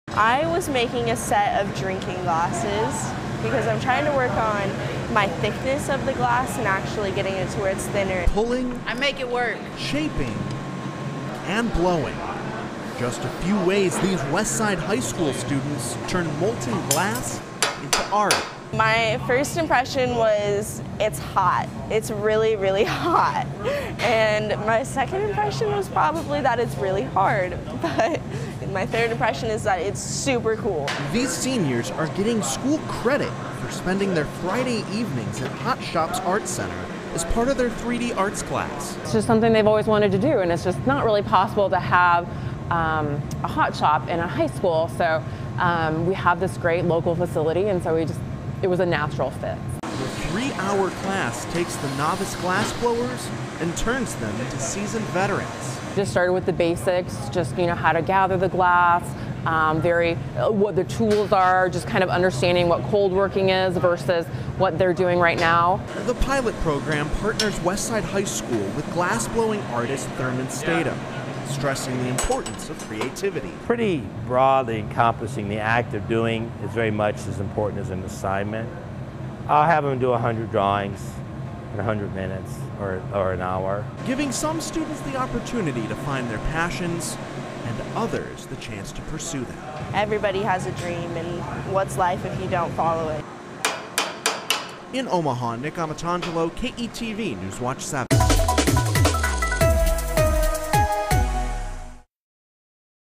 Westside seniors learn glassblowing at Hot Shops Art Studio
Most teenagers would likely put taking classes low on a list of how they could spend a Friday night, but a group of Westside High School seniors have spent Friday nights this semester learning a new skill and earning credit for their work.